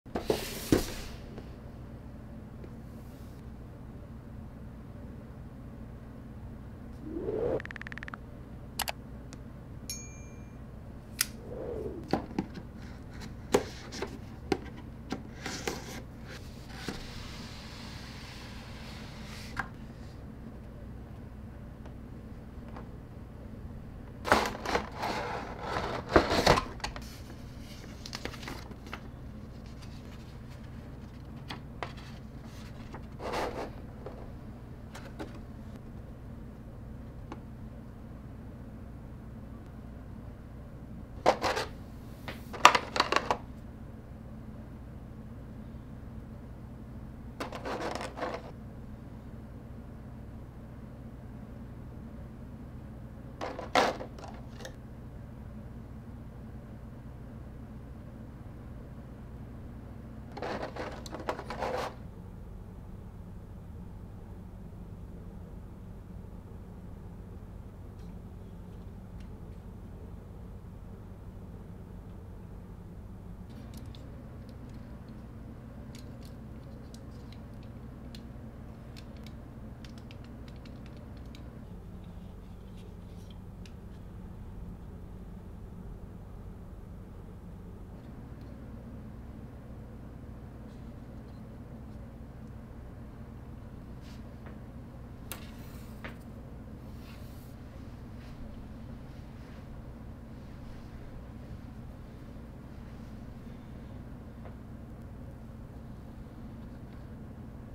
HALO INFINITE 1:12 MASTER CHIEF MJONIR MK VI GEN 3 | ASMR | 4K
🔥Social Medias 🔥

    🤝 Looking for Partners! 🤝

🎮 Controller Gang!

00:00 Intro
00:45 Box Reveal
01:31 Documents
01:40 Statue Assembly
03:25 B Roll
03:51 Final Thoughts

Show my teammates some love on Social!